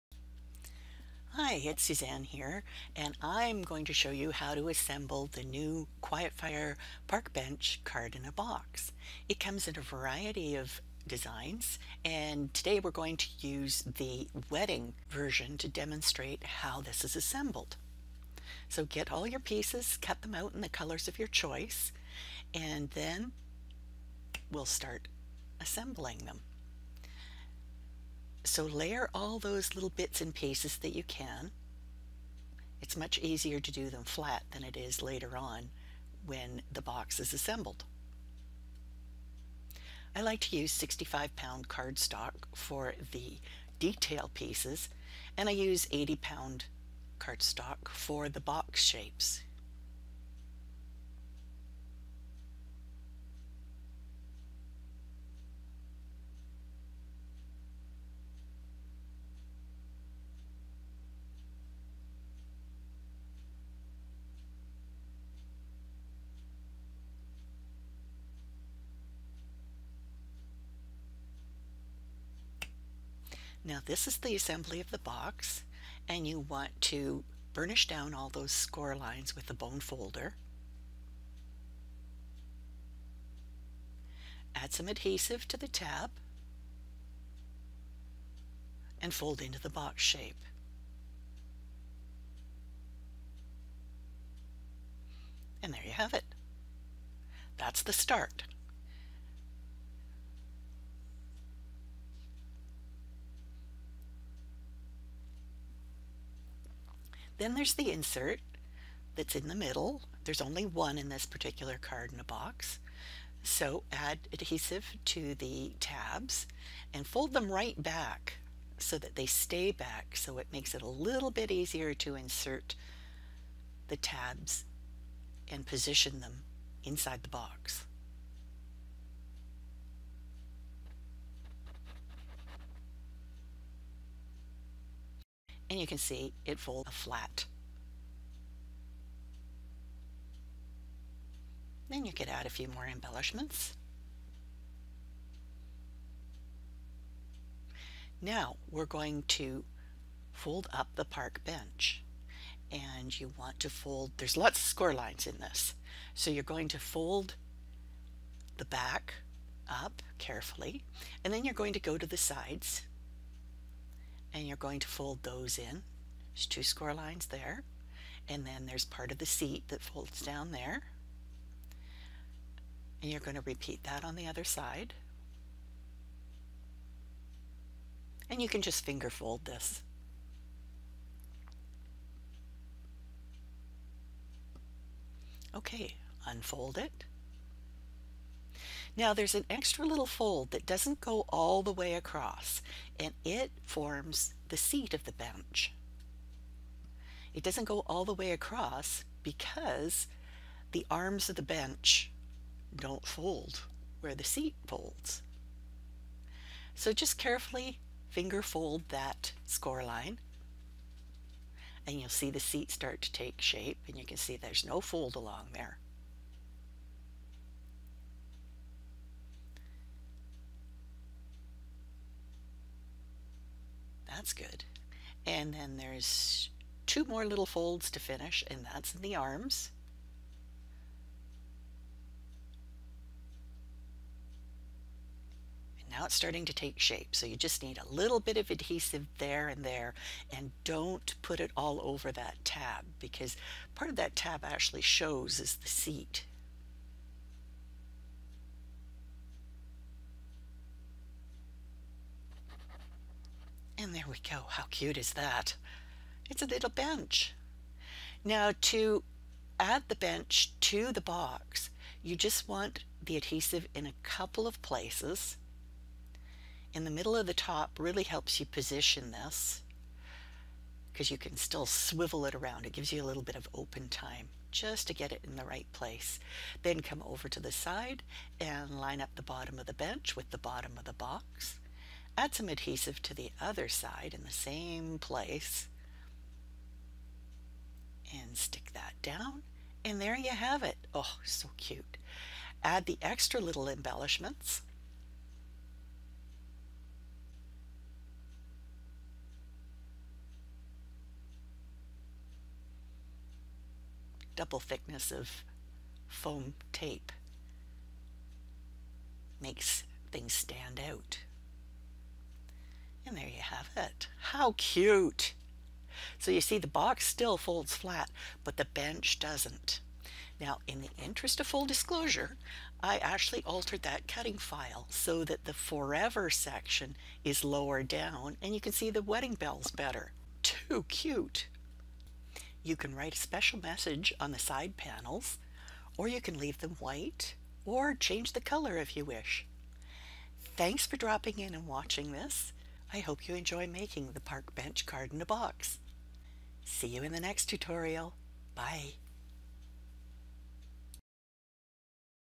Assembling the Quietfire Park Bench Card-in-a-Box
Here's a really fun addition to your Card-in-a-Box collection! This is the Wedding version, but there are a whole bunch more themes!  You can use all the same designs or mix and match! Here is a quick video tutorial to help you get started!  Want this design to go completely flat?

These Park Bench Card-in-a-Box designs are available on my Silhouette Design Store.